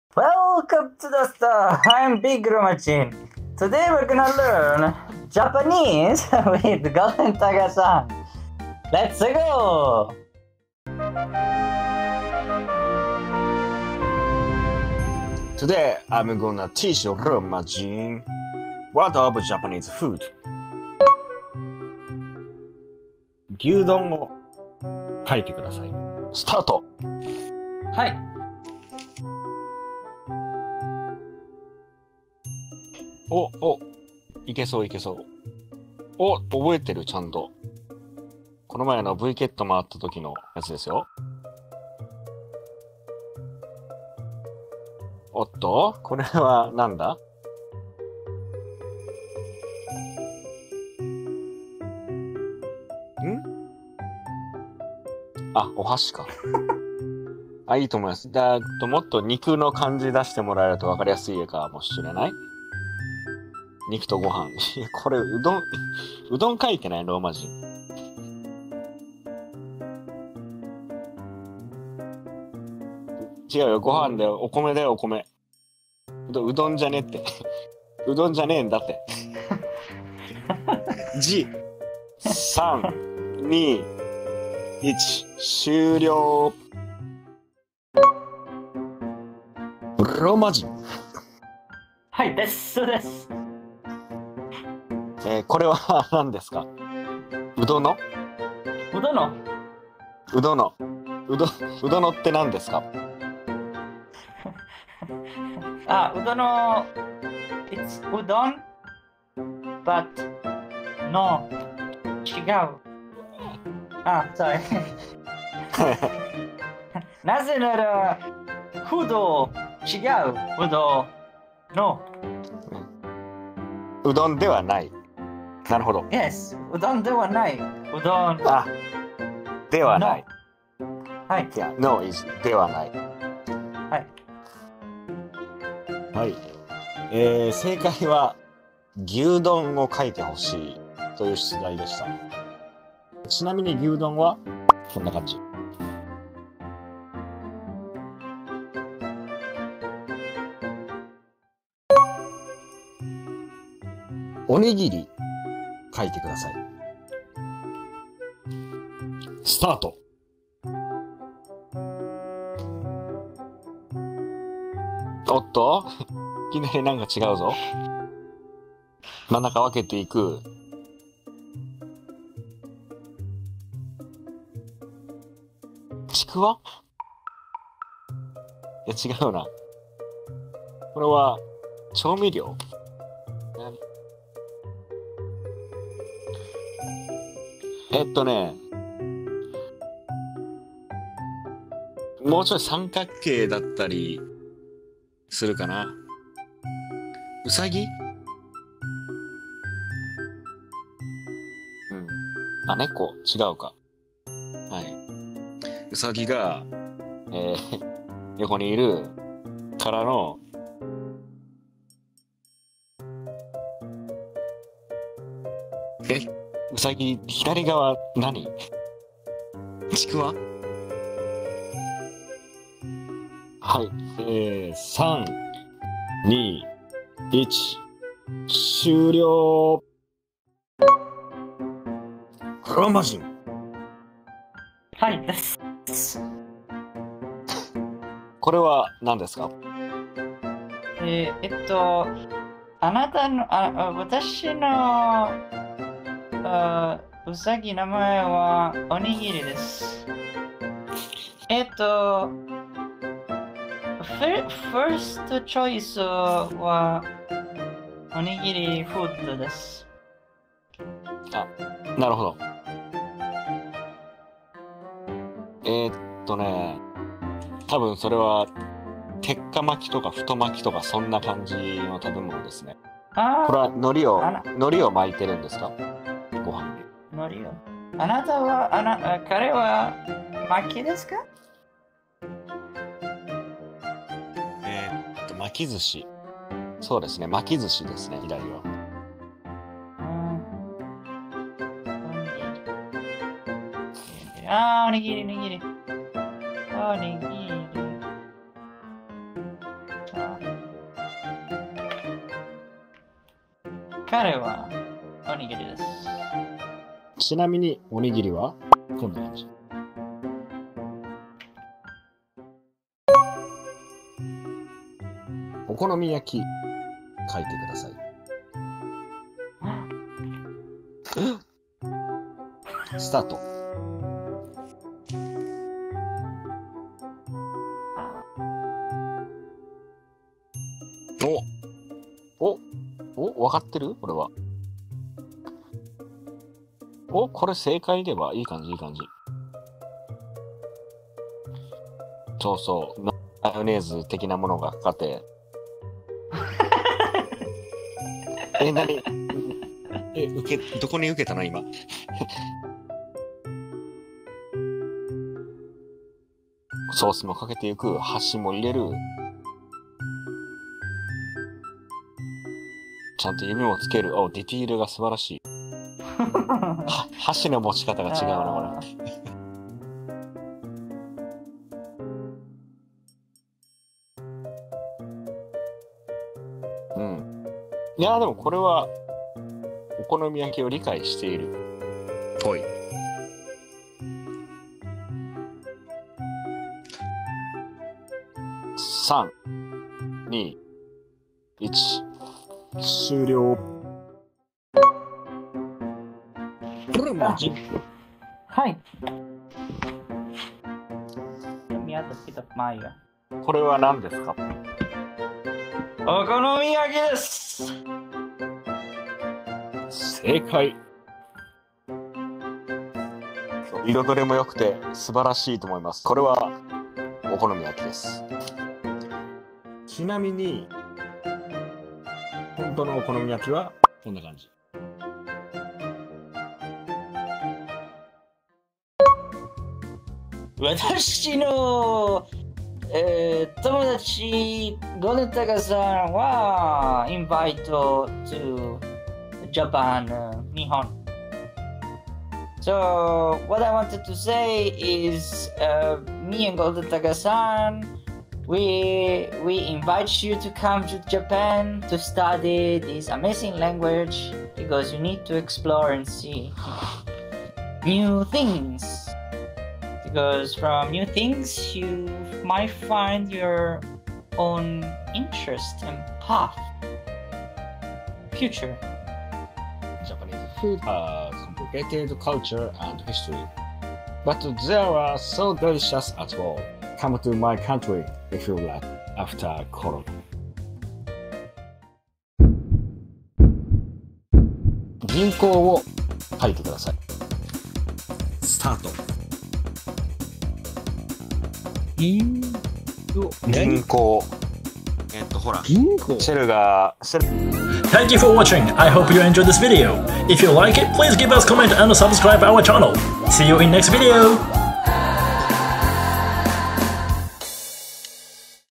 #392 Japanese lesson in VRCHAT vol.35 - お絵描き日本語リスニング
Golden Takasan and Romajin play with Japanese listening and drawing.